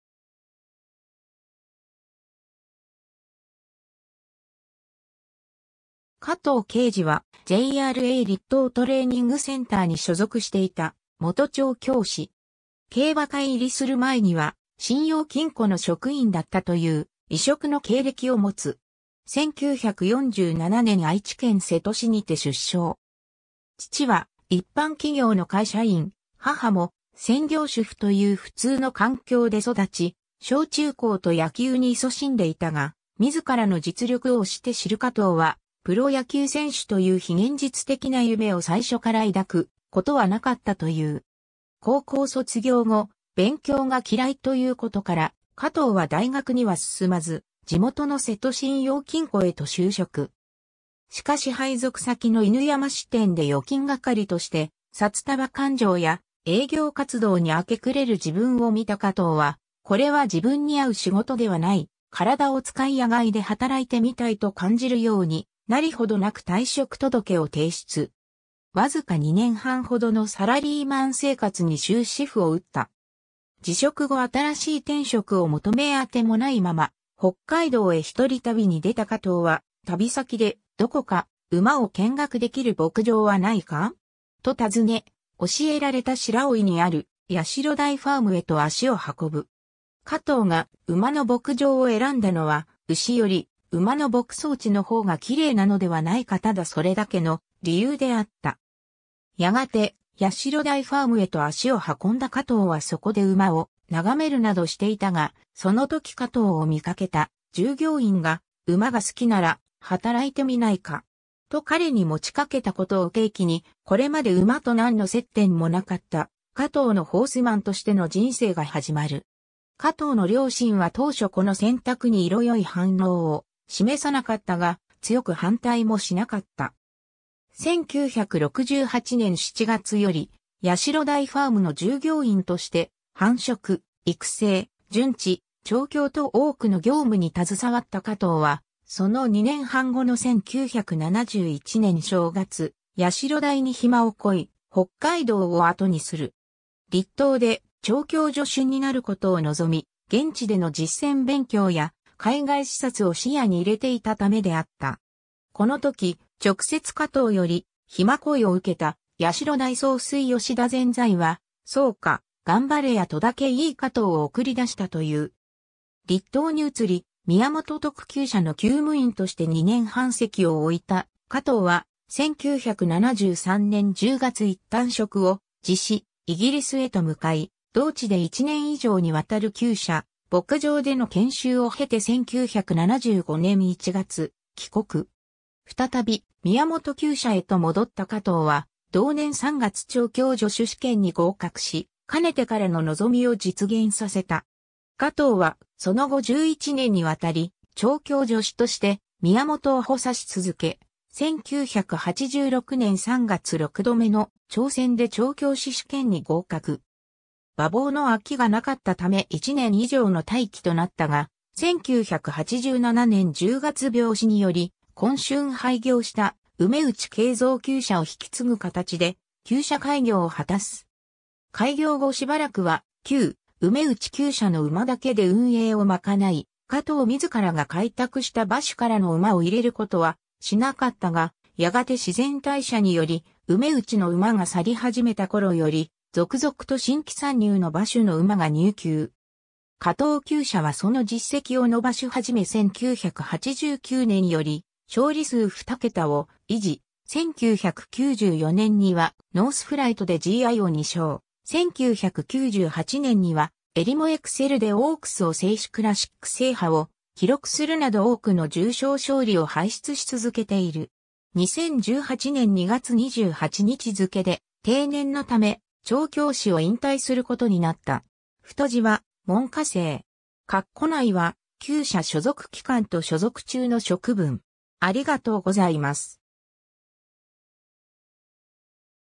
加藤敬二 (競馬)
加藤 敬二（かとう けいじ、1947年4月21日 - ）はJRA栗東トレーニングセンターに所属していた元調教師。
競馬界入りする前には信用金庫の職員だったという、異色の経歴を持つ。
 1947年愛知県瀬戸市にて出生。
父は一般企業の会社員・母も専業主婦という普通の環境で育ち、小中高と野球に勤しんでいたが自らの実力を推して知る加藤はプロ野球選手という非現実的な夢を最初から抱くことはなかったという。
 高校卒業後、勉強が嫌いということから加藤は大学には進まず地元の瀬戸信用金庫へと就職。
しかし配属先の犬山支店で預金係として札束勘定や営業活動に明け暮れる自分を見た加藤は「これは自分に合う仕事ではない、体を使い野外で働いてみたい」と感じるようになりほどなく退職届を提出。
僅か2年半ほどのサラリーマン生活に終止符を打った。
 辞職後新しい天職を求めアテも無いまま北海道へ一人旅に出た加藤は旅先で「どこか馬を見学できる牧場はないか？」と尋ね、教えられた白老にある社台ファーム（現在の白老ファーム）へと足を運ぶ。
加藤が馬の牧場を選んだのは「牛より馬の牧草地のほうが綺麗なのではないか」ただそれだけの理由であった。
 やがて社台ファームへと足を運んだ加藤はそこで馬を眺めるなどしていたが、その時加藤を見かけた従業員が「馬が好きなら、働いてみないか?」と彼に持ちかけたことを契機に、これまで馬と何の接点も無かった加藤のホースマンとしての人生が始まる。
加藤の両親は当初この選択に色よい反応を示さなかったが、強く反対もしなかった。
 1968年7月より社台ファームの従業員として繁殖・育成・馴致・調教と多くの業務に携わった加藤はその2年半後の1971年正月、社台に暇を請い北海道を後にする。
栗東で調教助手になることを望み、現地での実践勉強や海外視察を視野に入れていたためであった。
この時直接加藤より暇請いを受けた社台総帥吉田善哉は「そうか、頑張れや」とだけいい加藤を送り出したという。
 栗東に移り、宮本悳厩舎の厩務員として2年半籍を置いた加藤は、1973年10月一旦職を辞しイギリスへと向かい、同地で1年以上に渡る厩舎・牧場での研修を経て1975年1月帰国。
再び宮本厩舎へと戻った加藤は同年3月調教助手試験に合格し、かねてからの望みを実現させた。
加藤はその後11年にわたり調教助手として宮本を補佐し続け、1986年3月6度目の挑戦で調教師試験に合格。
馬房の空きが無かったため1年以上の待機となったが、1987年10月病死により今春廃業した梅内慶蔵厩舎を引き継ぐ形で厩舎開業を果たす。
 開業後暫くは旧梅内厩舎の馬だけで運営を賄い、加藤自らが開拓した馬主からの馬を入れることはしなかったがやがて自然代謝により梅内の馬が去り始めたころより、続々と新規参入の馬主の馬が入厩。
加藤厩舎はその実績を伸ばし始め1989年より勝利数2桁を維持、1994年にはノースフライトでGIを2勝、1998年にはエリモエクセルでオークスを制しクラシック制覇を記録するなど多くの重賞勝利を輩出し続けている。
 2018年2月28日付けで定年の為、調教師を引退することになった。
 ※太字は門下生。
括弧内は厩舎所属期間と所属中の職分。